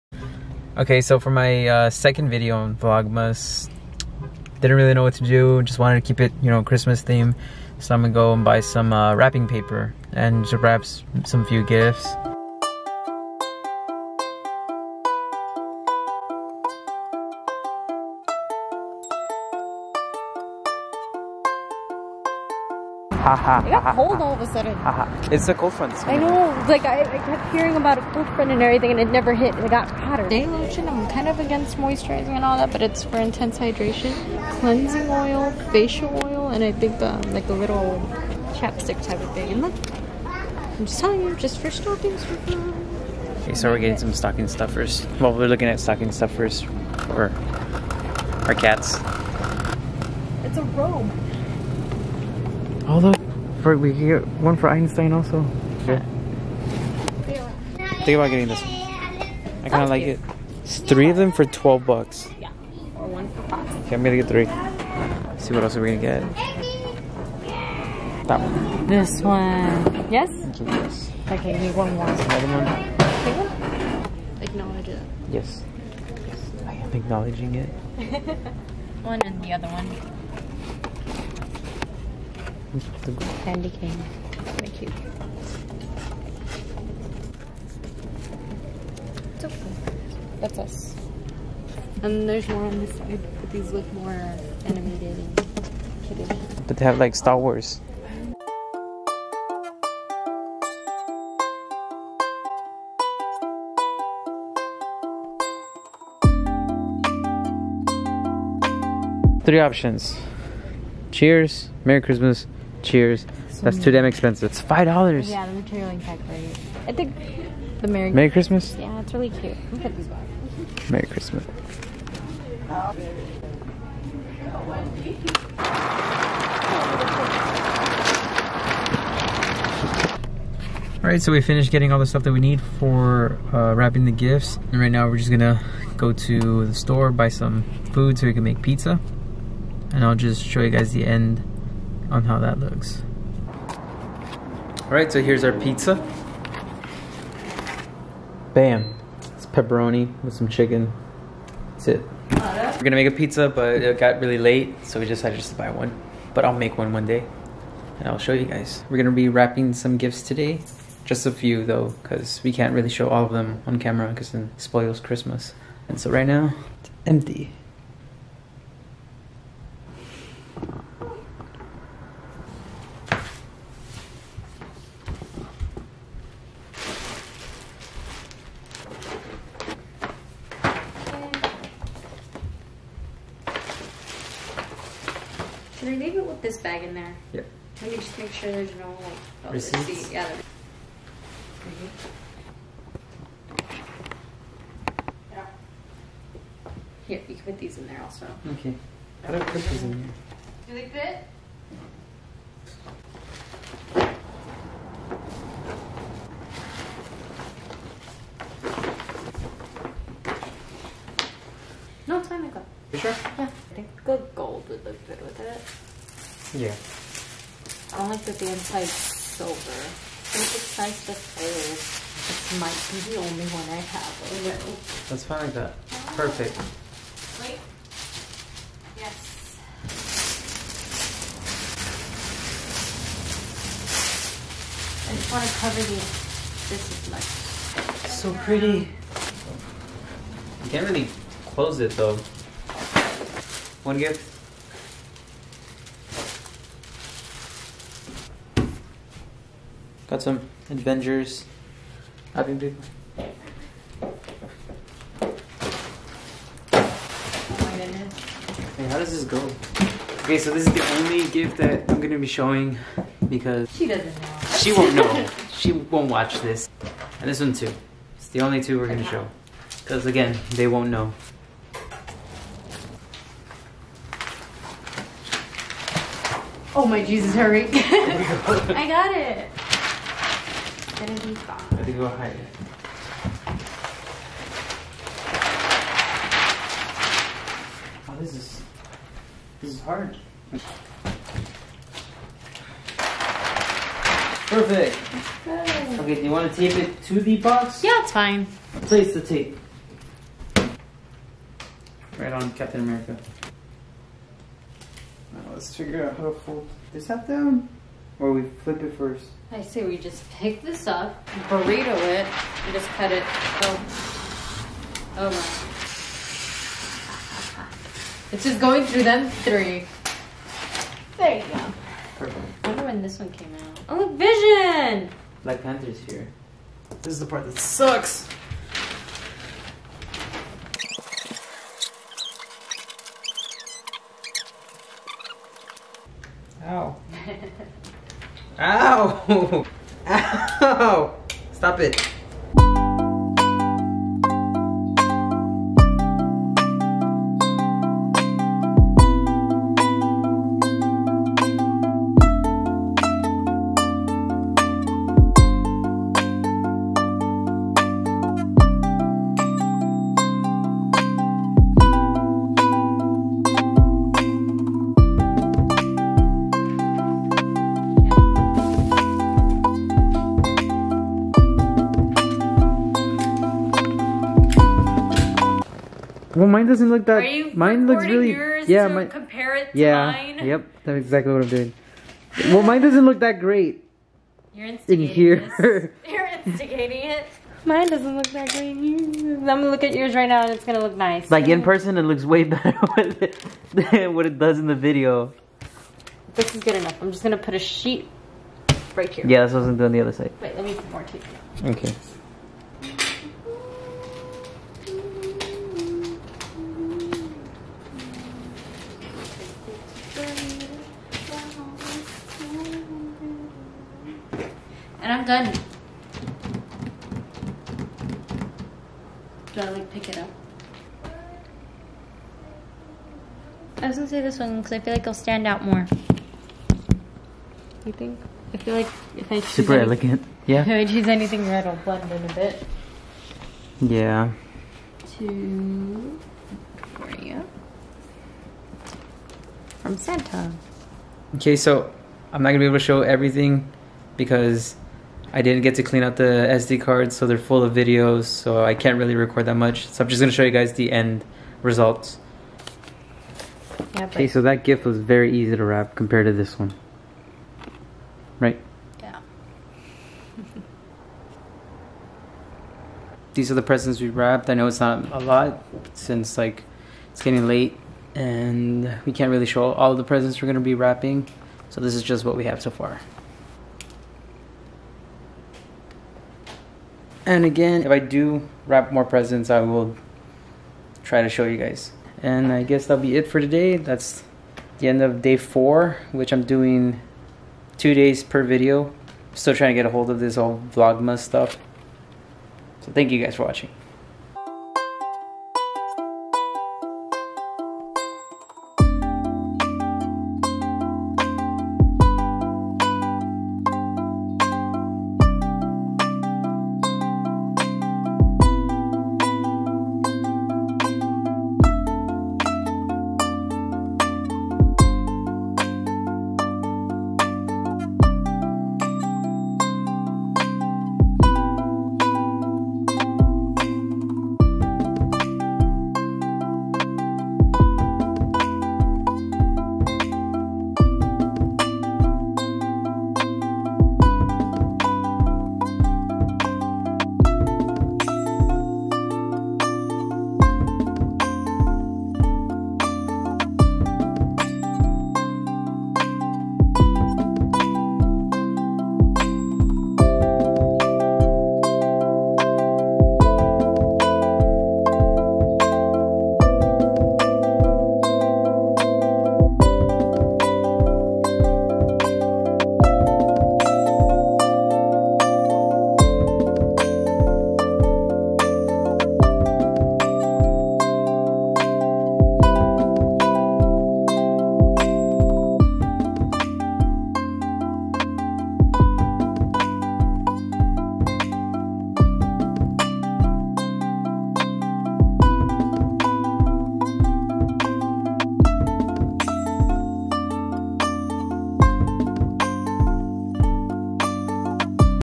Vlogmas Part 2! Days 3-4
Some Christmas shopping and wrapping gifts!